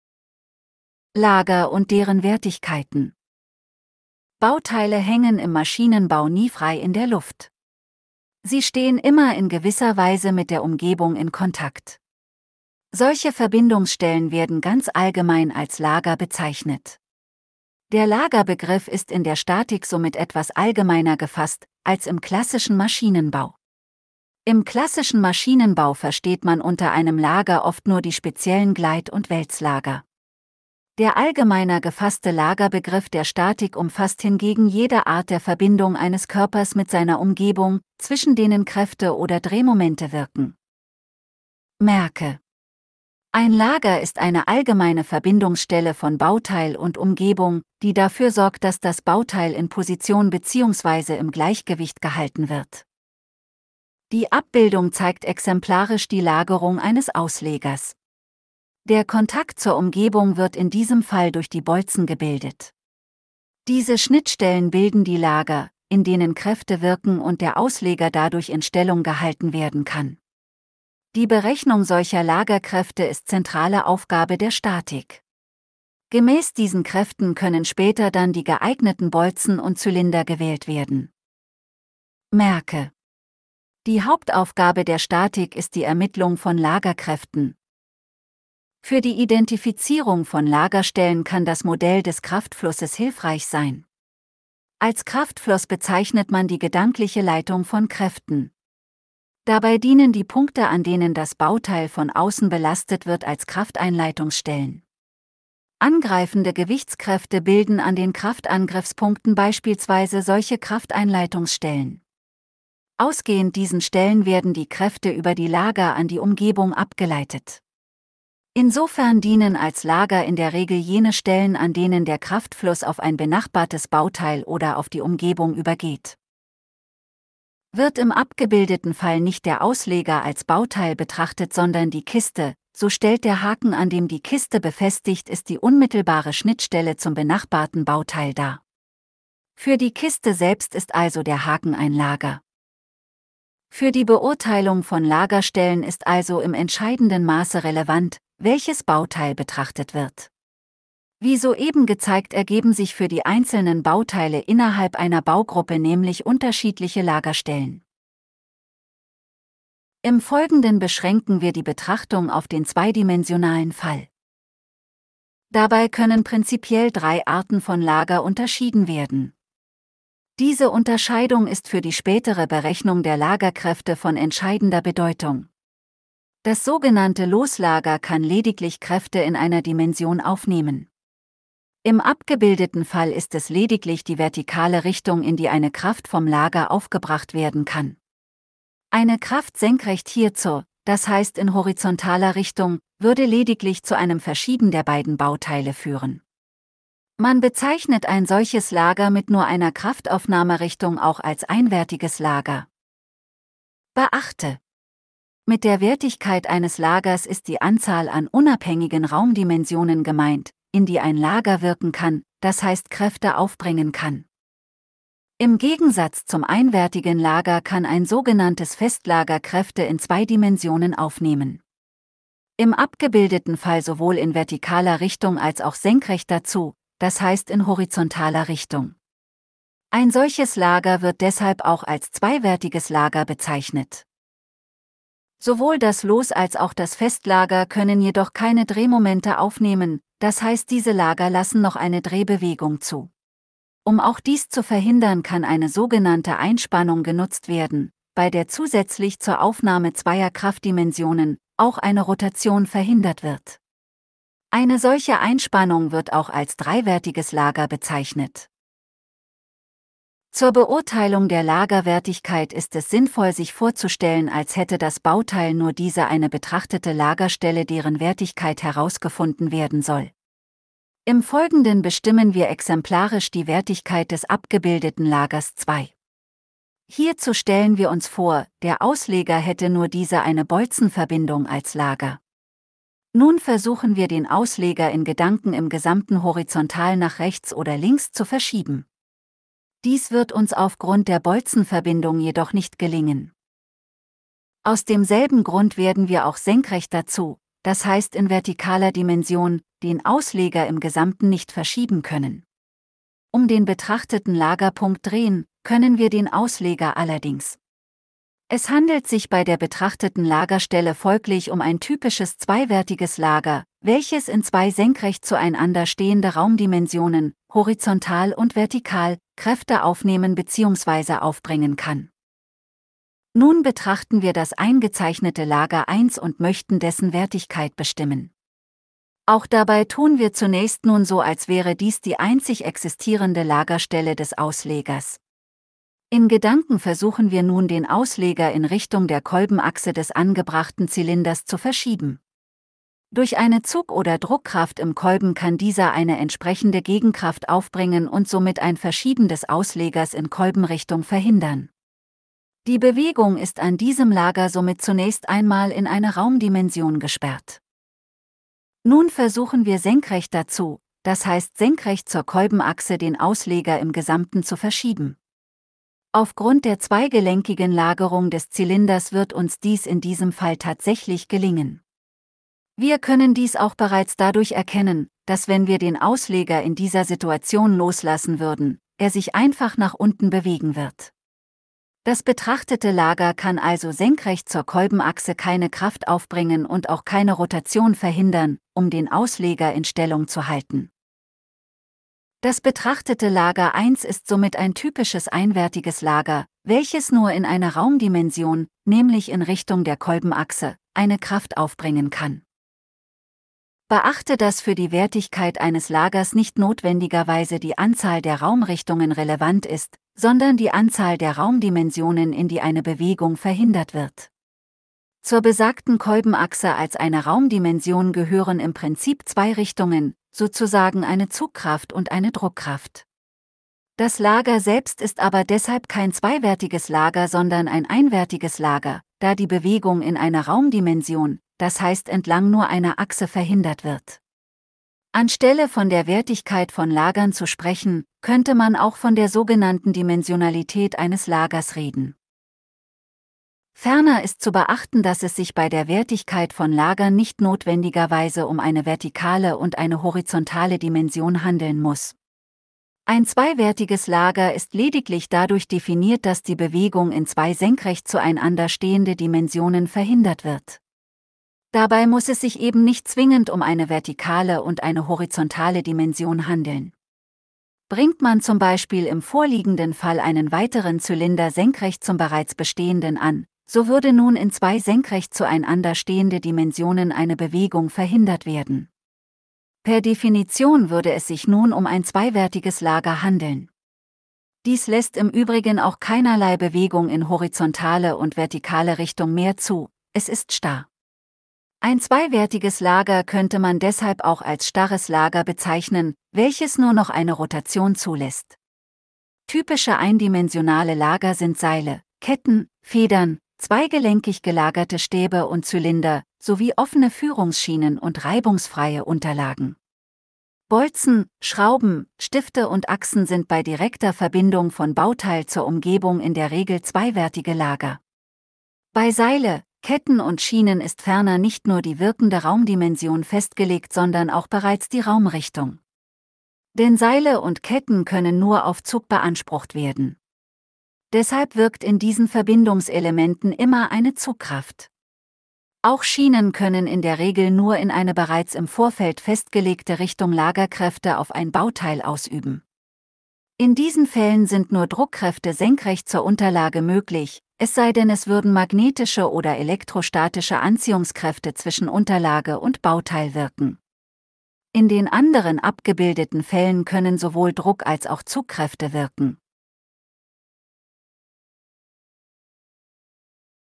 Lager und deren Wertigkeiten
In diesem Video beschäftigen wir uns mit dem allgemeinen Begriff des Lagers und der Wertigkeiten von Lagern.

00:00 Begriff des Lagers in der Statik
02:39 Ein-, zwei- und dreiwertige Lager
04:20 Bestimmung der Lagerarten
08:19 Weitere Beispiele